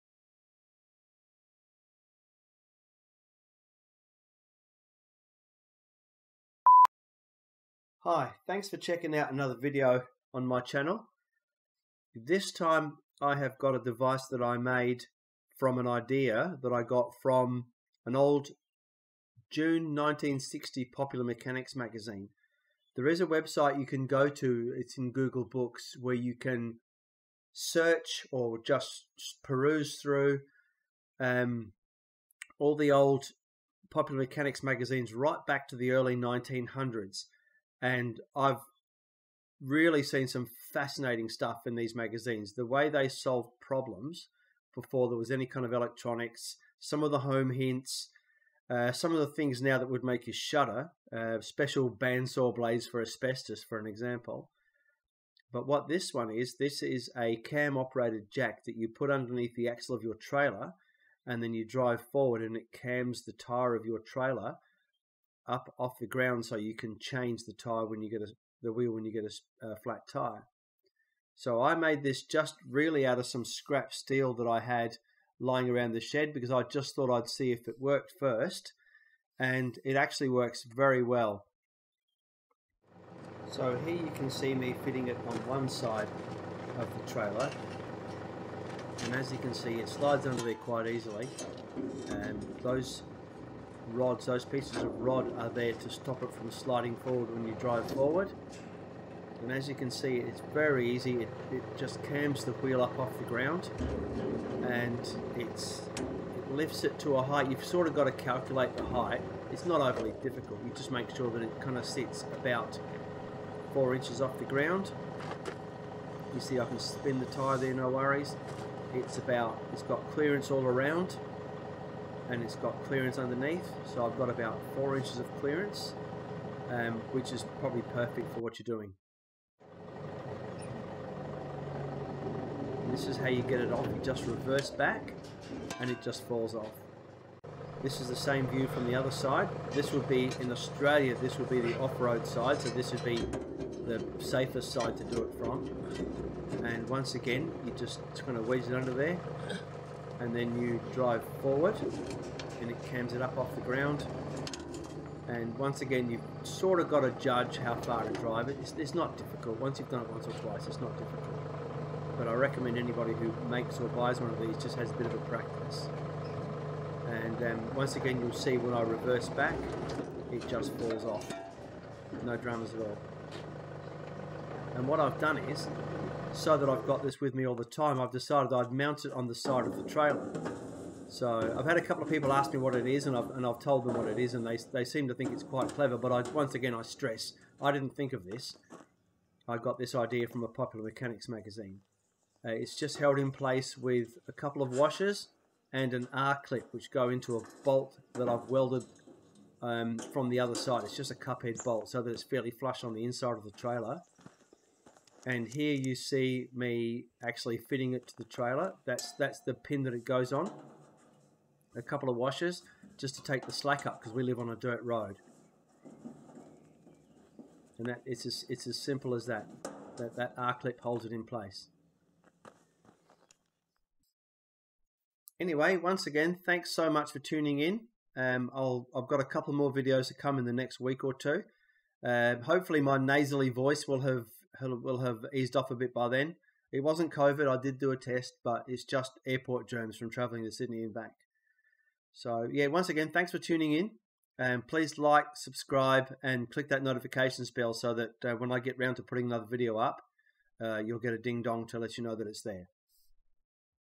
Cam style trailer axle jack.
This is something I made after spotting it in an old Popular Mechanics magazine. It is used for raising the trailer axle off the ground to change a wheel if you get a flat tyre, it has no moving parts and as you'll see from the video, it is very strong, and much easier than fiddling with a jack on the side of the road. The Popular Mechanics magazines can be found
I hope you find them as interesting as I have and a great way to fill in time if you're stuck somewhere with nothing to read.